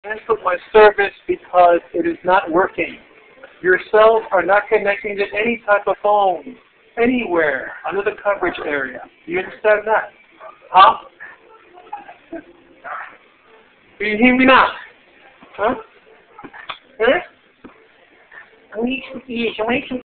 can u hear me now(verizon wireless guy gets his ear bitten)
verizon wireless commercial guy gets his ear bitten